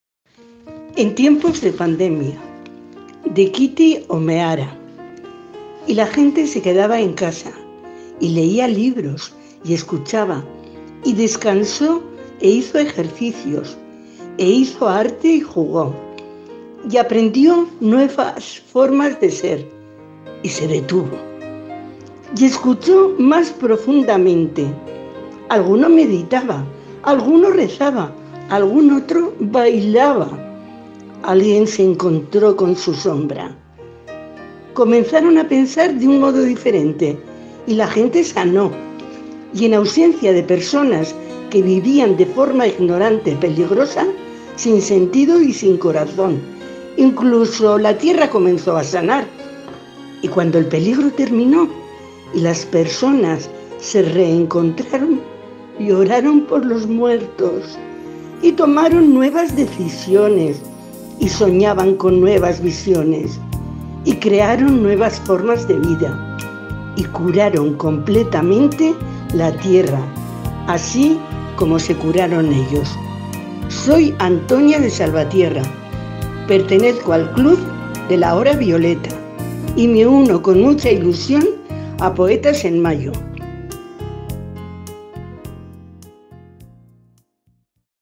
LAIAPlazara - LAIArtea - PoetasEnMayo - Antonia
LAIAeskola, Irakurle-klub feministak eta poesia / LAIAeskola Clubs de lectura feminista y poesía

Antonia  Club de Lectura Feminista "La hora violeta" de Arabako Lautada.